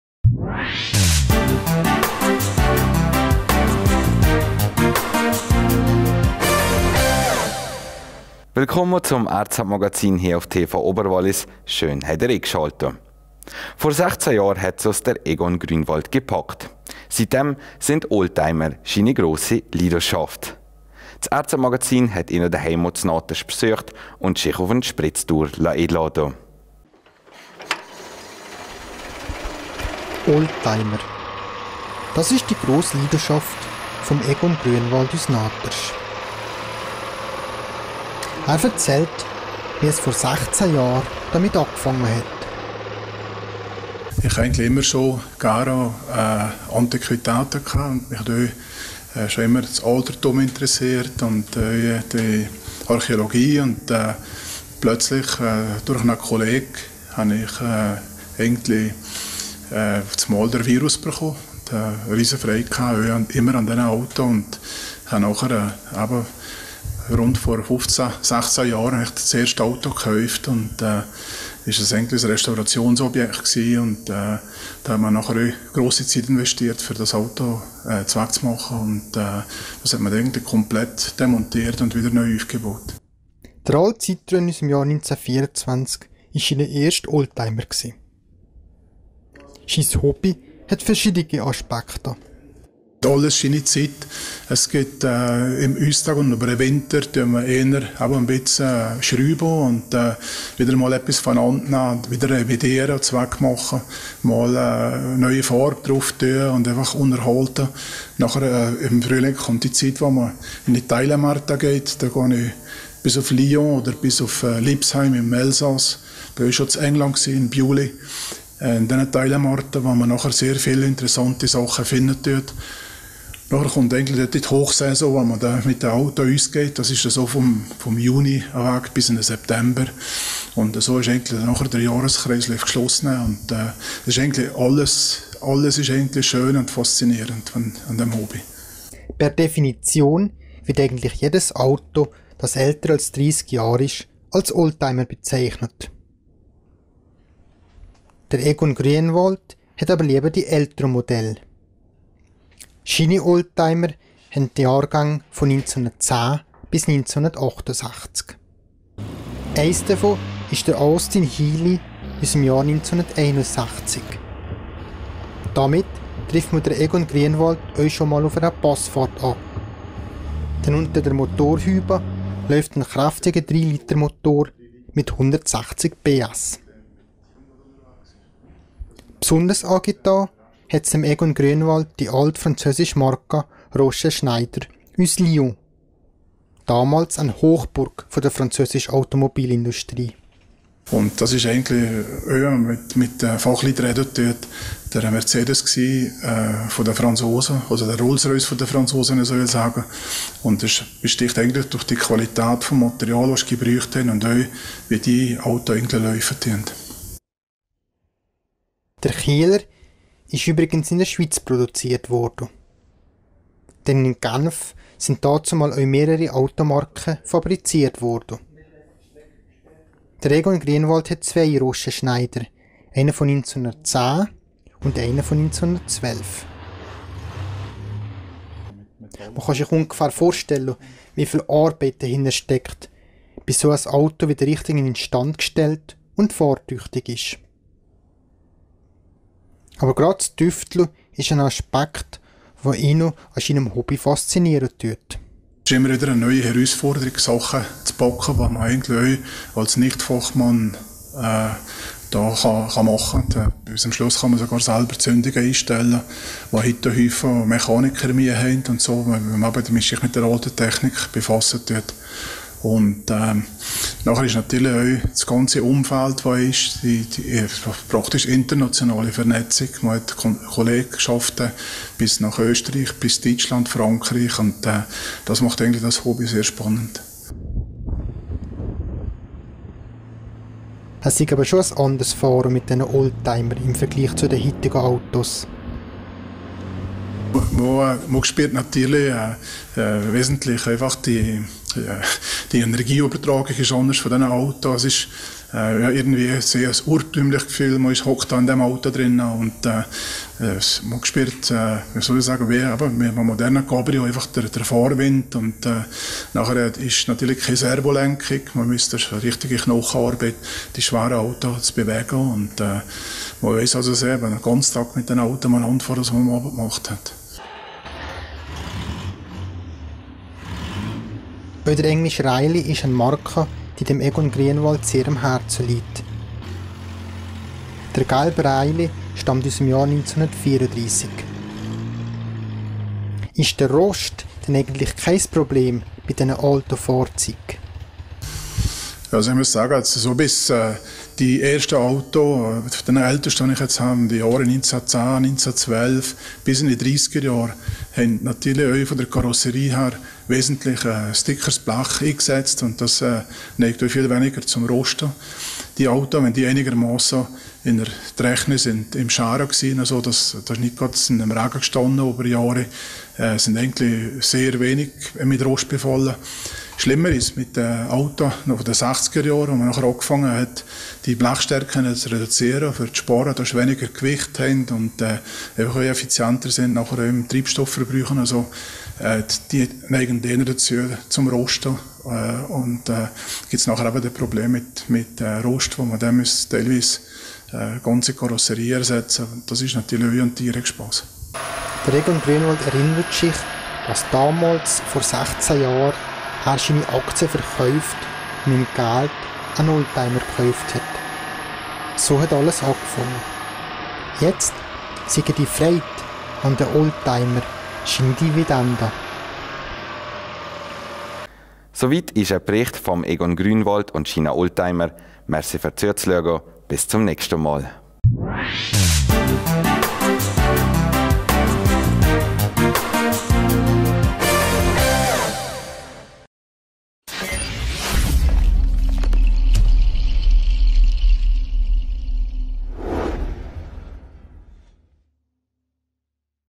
Leidenschaft Oldtimer
Vor 16 Jahren hat es Egon Grünwald gepackt. Seitdem sind Oldtimer seine Passion. Das RZ-Magazin hat ihn zu Hause in Naters besucht  und auf einer kleinen Spritztour begleitet.